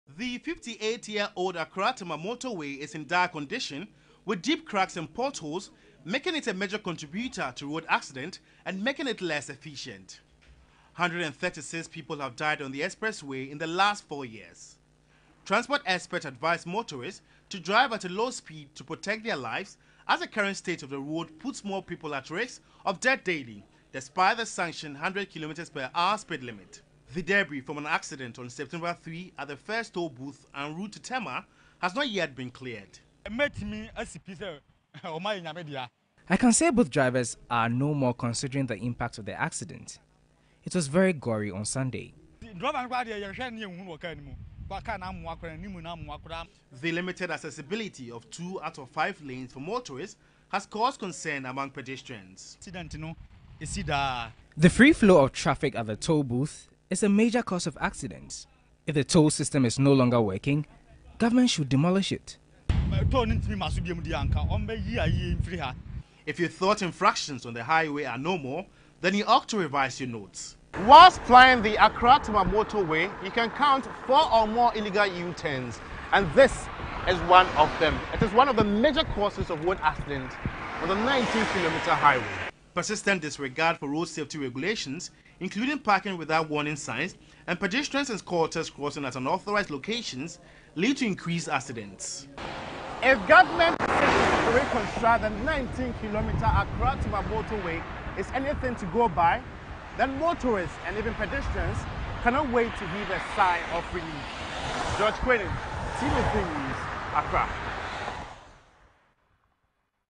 Accra-Tema motorway: State of highway a threat to motorists
Motorists will be risking their lives if they maintain the 100km per hour on the Accra-Tema motorway due to its current state.

The highway has been ravaged by deep potholes and cracks coupled with persistent infractions, a major contributory factor to road accidents on the 19km stretch.